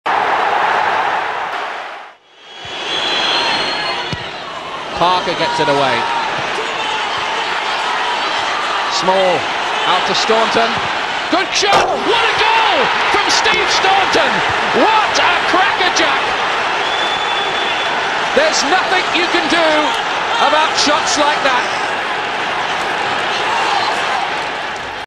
Steve Staunton vs Man United
Staunton scores a ripper for Aston Villa against United at Old Trafford